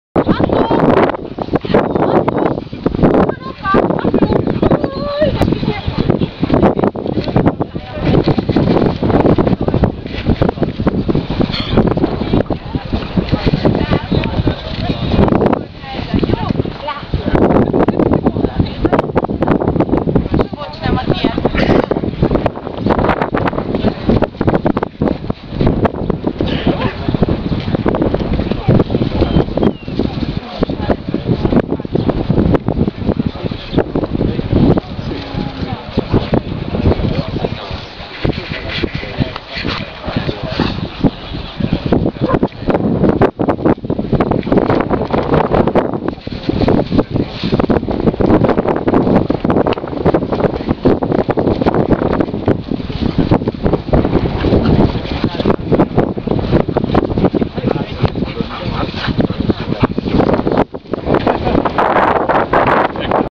Zidan von der Janvari 1
Zidan von der Janvari on the first place Tito Earl Antonius on the second place on MARK Rottweiler Show 09Th April 2011 Judge was Frank Hedtke ADRK. Tito was the open class winner.